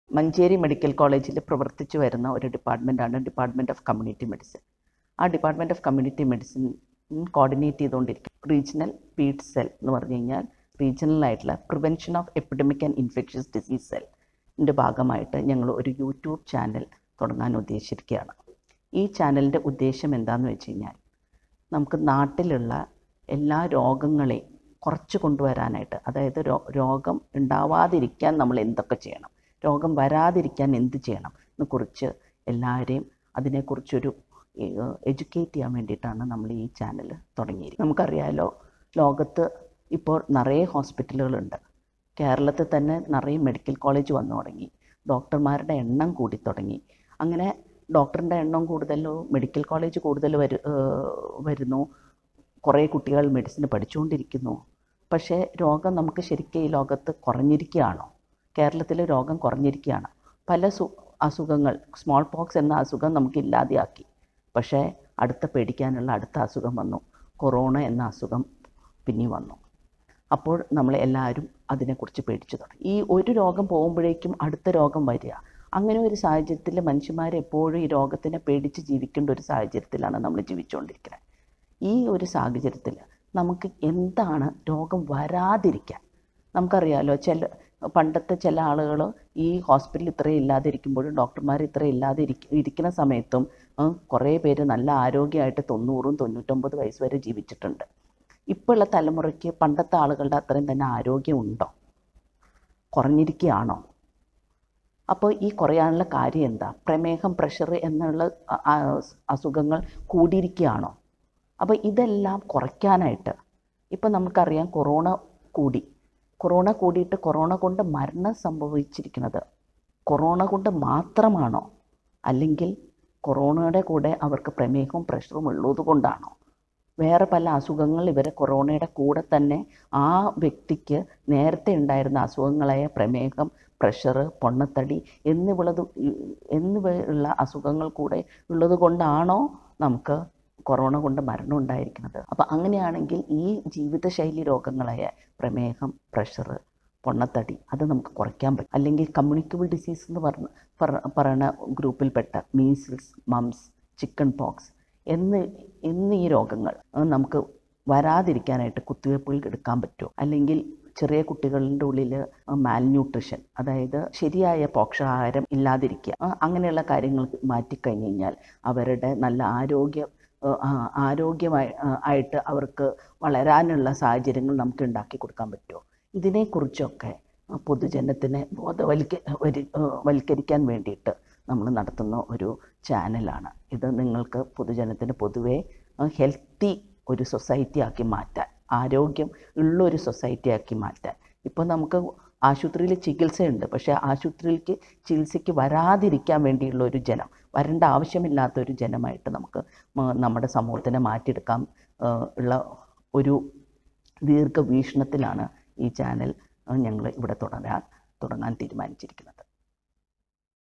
Dr. Nileena Koshy, Coordinator, RPEID Cell, GMC Manjeri
This video is an attempt by the Regional Prevention of Epidemic and Infectious Disease Cell, GMC Manjeri, to connect with people to share health related knowledge and information. Reaching out to people directly during this pandemic is not feasible, therefore this is an humble effort to fulfil the laid objectives.